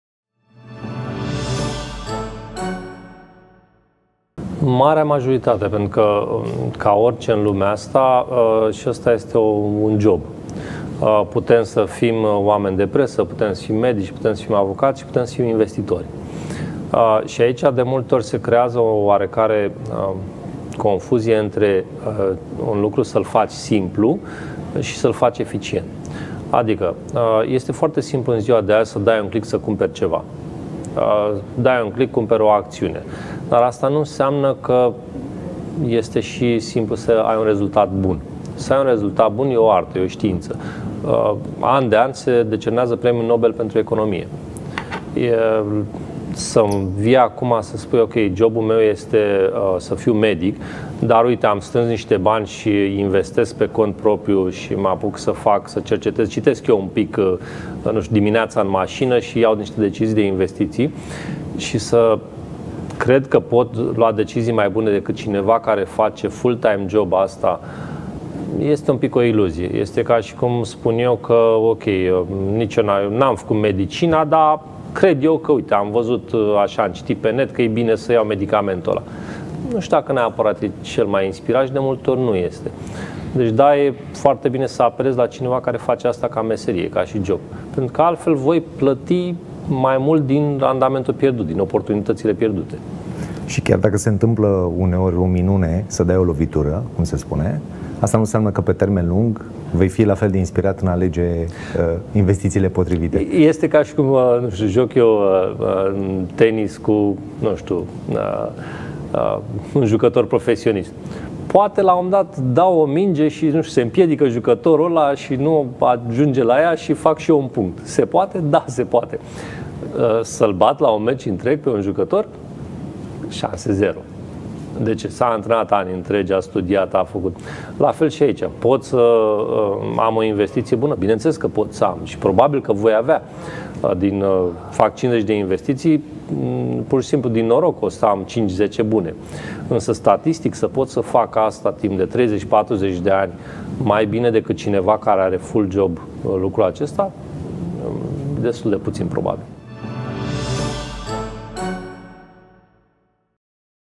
Este bine să apelăm la profesioniști pentru a investi
Când banii se depreciază, se uzează, toată munca și energia pe care ai investit-o pentru a-i dobândi se irosește, se duce printre degete. Despre asta este proiectul Economisește inteligent. Un proiect susținut de AAF (Asociația Administratorilor de Fonduri).